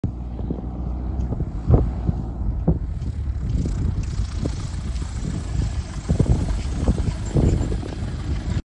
5 to 7 mph wind and this Husky lands short!
A beautiful combination of short field performance with long range and high speed capabilities!!!

@ak_husky_rep (instagram)